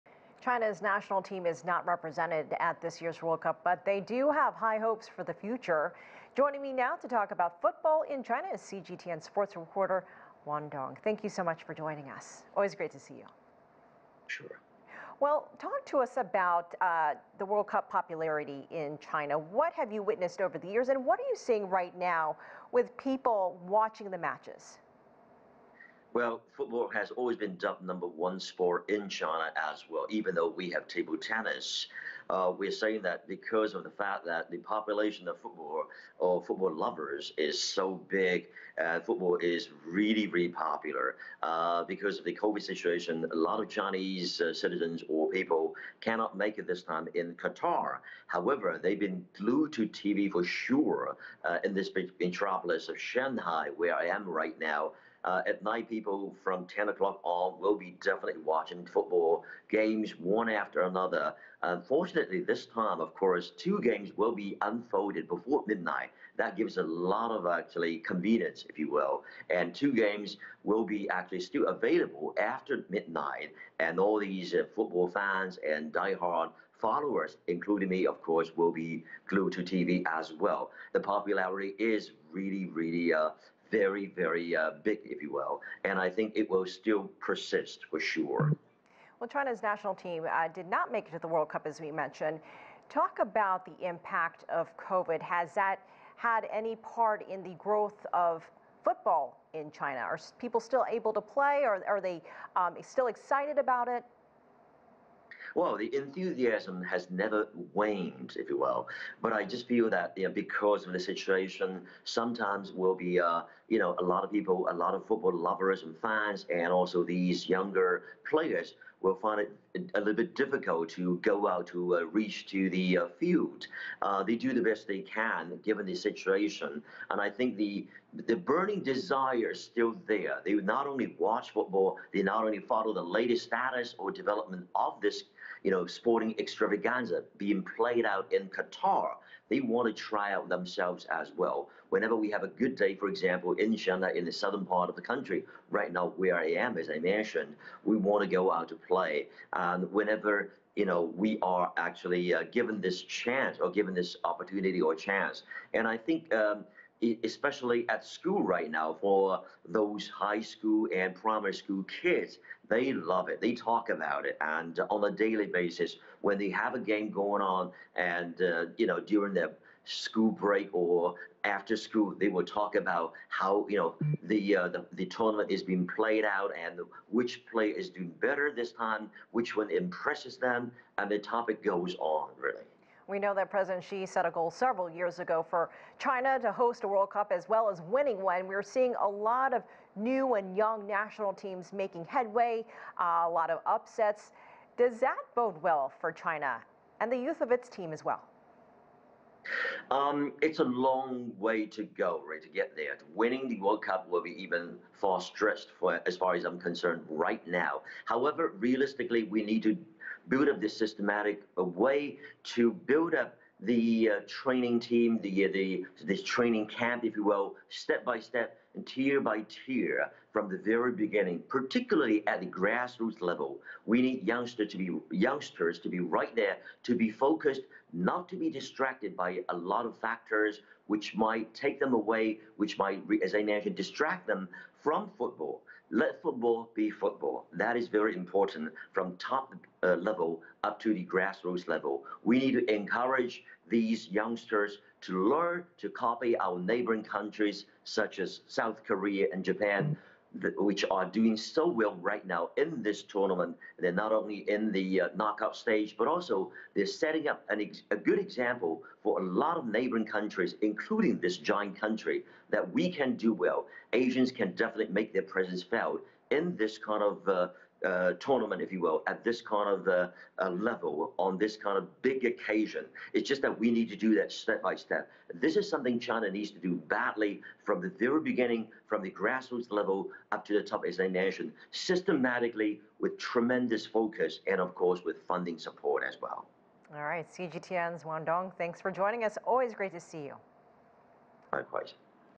Wang Dong on China soccer ambitions
CGTN’s Elaine Reyes speaks to Wang Dong. He’s a sports reporter for CGTN. They discuss China’s ambitions and the goal to reach the world cup stage.